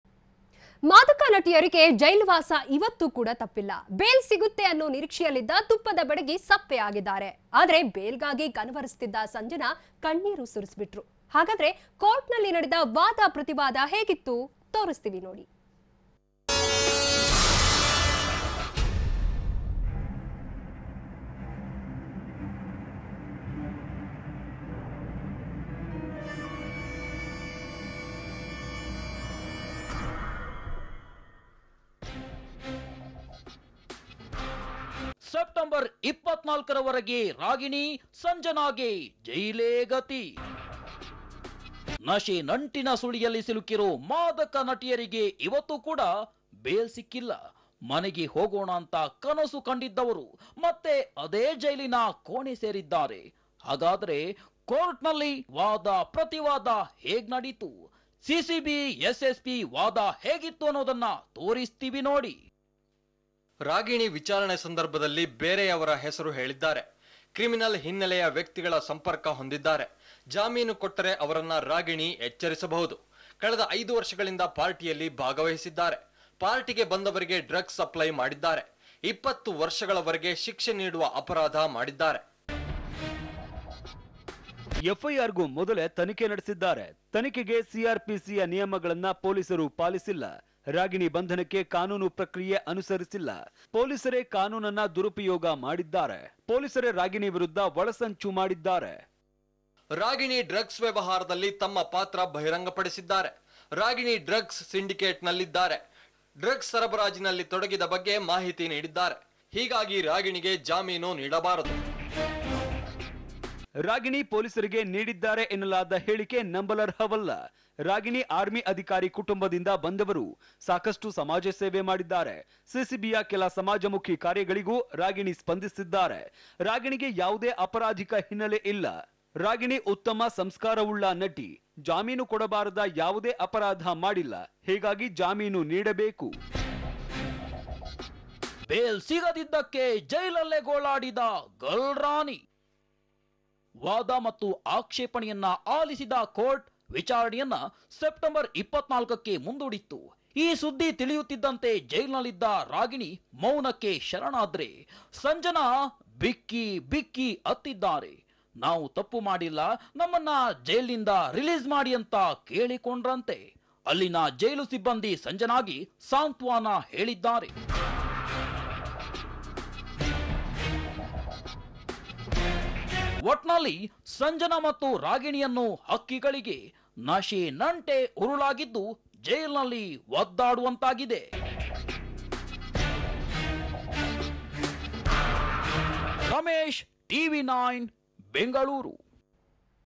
'ಗಿಣಿ ಸೈಲೆಂಟ್ ಗಲ್ರಾನಿ ವೈಲೆಂಟ್': Sanjana & Ragini Will Spend 3 More Days In Jail
ರಾಗಿಣಿ, ಸಂಜನಾಗೆ ಇವತ್ತೂ ಸಿಗ್ಲಿಲ್ಲ ರಿಲೀಫ್ - ಇನ್ನೂ ಮೂರು ದಿನ ಜೈಲಿನಲ್ಲೇ ಲಾಕ್ - ಬೇಲ್ ಸಿಗದಿದ್ದಕ್ಕೆ ಗಲ್ರಾನಿ ಫುಲ್ ವೈಲೆಂಟ್

Actresses Ragini Dwivedi and Sanjana have three more days in jail. The judge, who is hearing the bail application, which is in NPS court today, has postponed the hearing to September 24. It is inevitable that the two will spend 3 more days in jail.